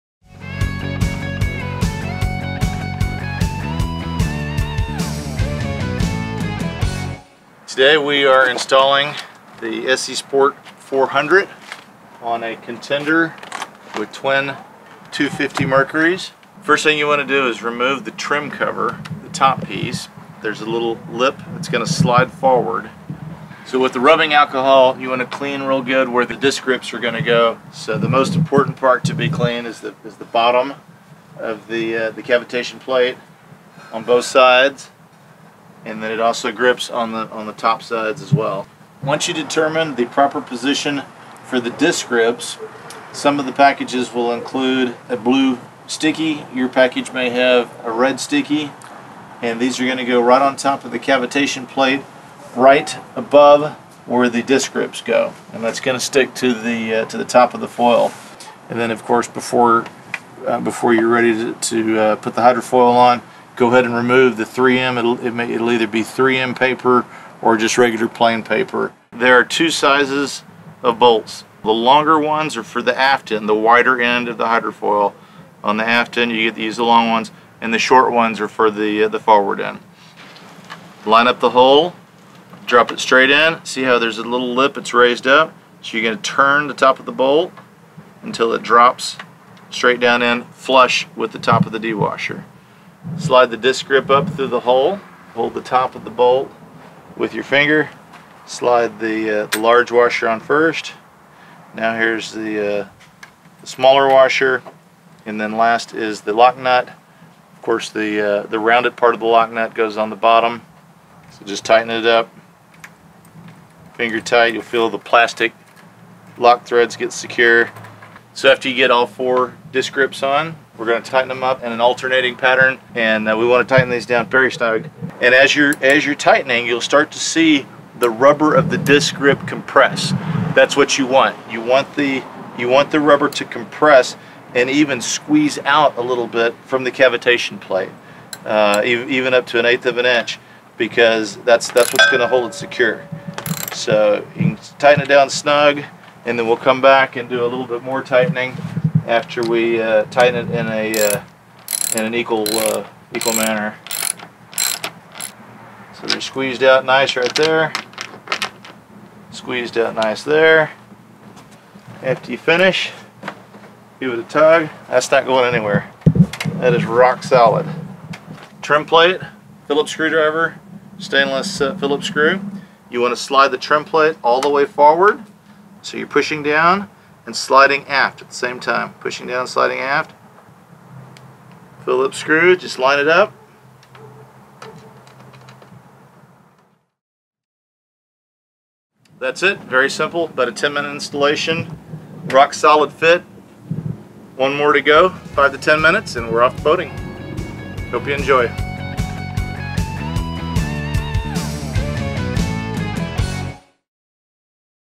SESport 400 Installation - iboats.com
Easy step by step instructions on installing the SESport 400 Hydrofoil.